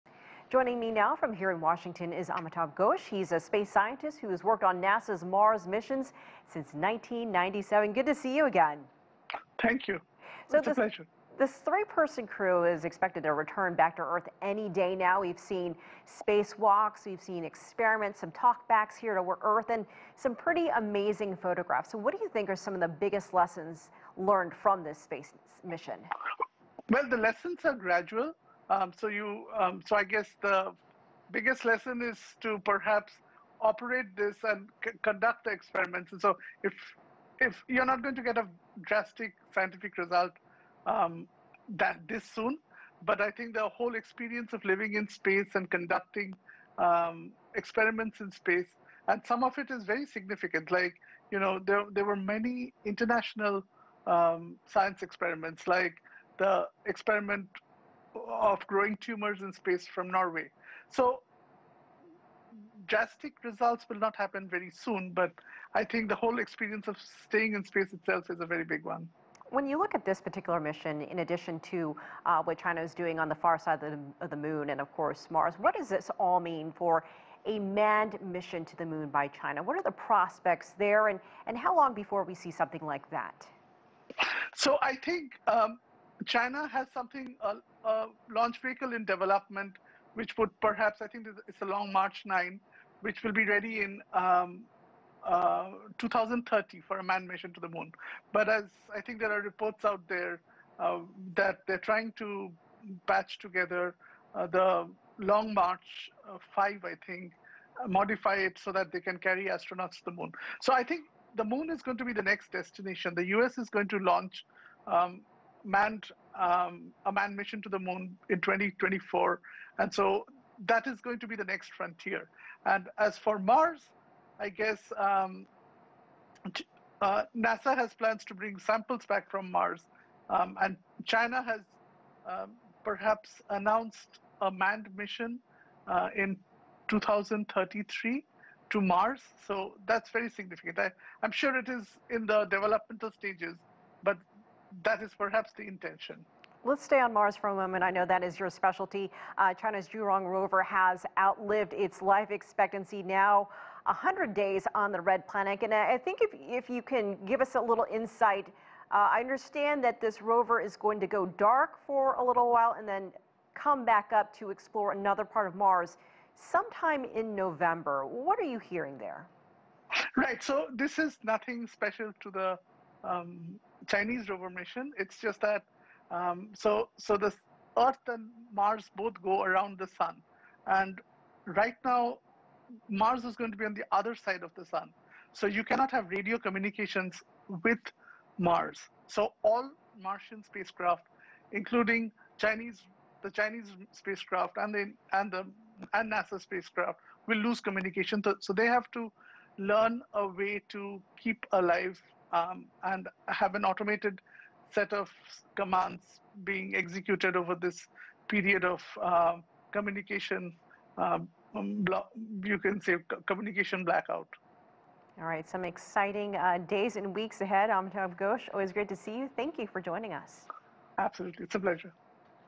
Amitabha Ghosh on the latest news about Chinas space program
CGTN spoke with Amitabha Ghosh about Chinese astronauts are headed back to Earth this month and an update on the China space program., He's a space scientist who's worked on NASA's Mars missions since 1997.